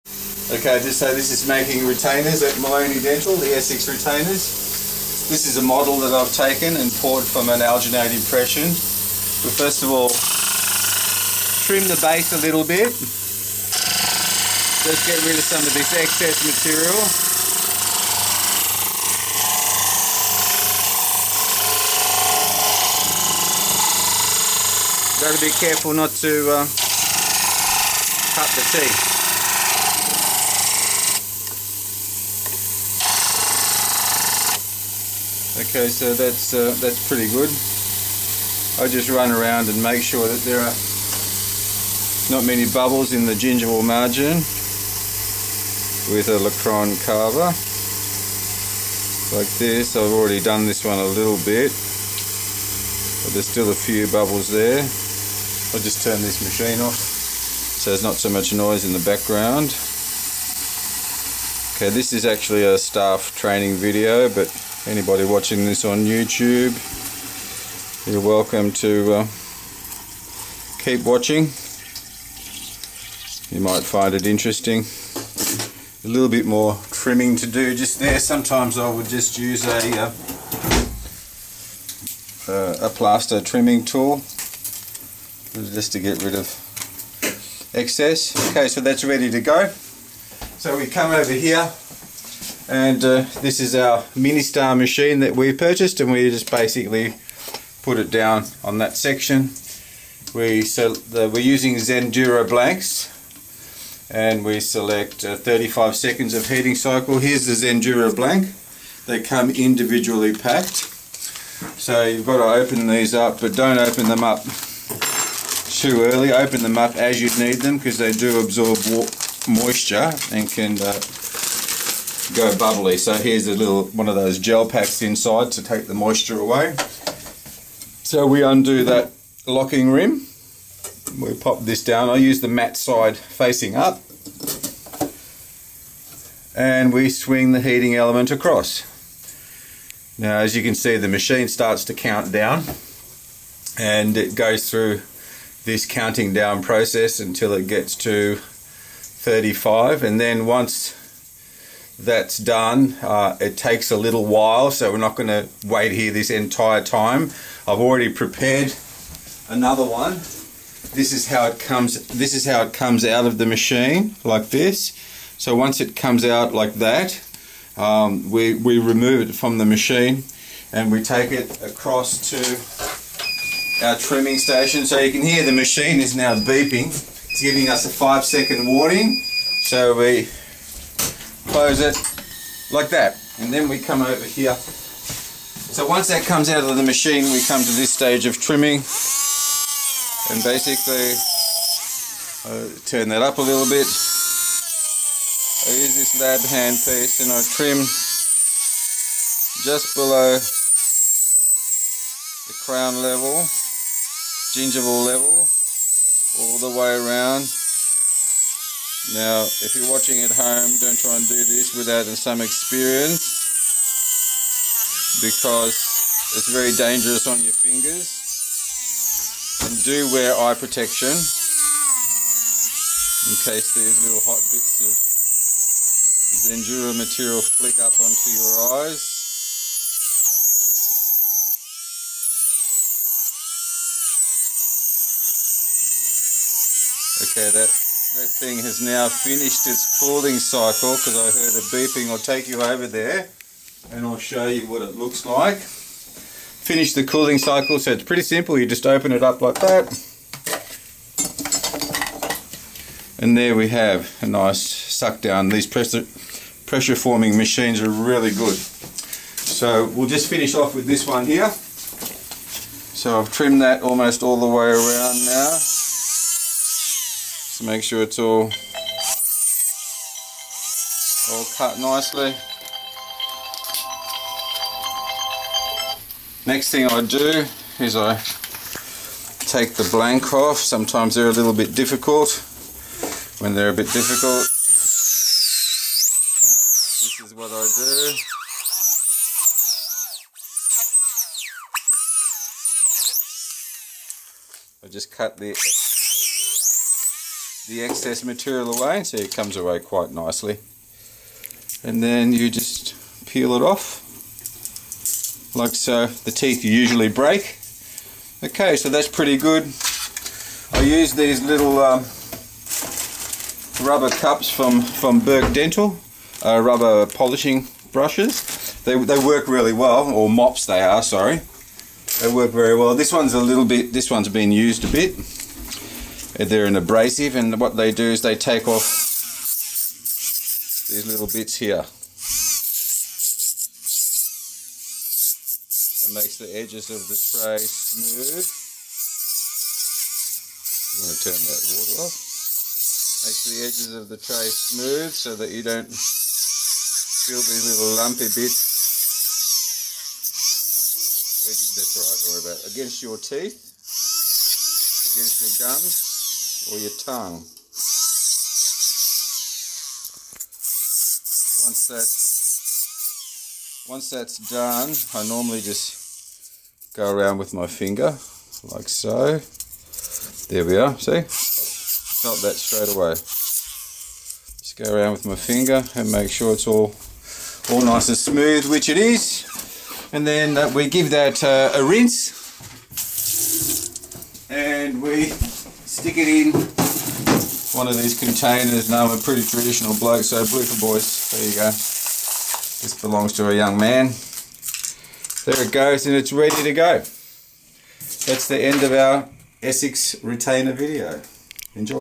Making Orthodontic Retainers
Video presentation on the fabrication of a clear orthodontic retainer using Zendura blanks. This video is intended for staff training purposes and for general public interest.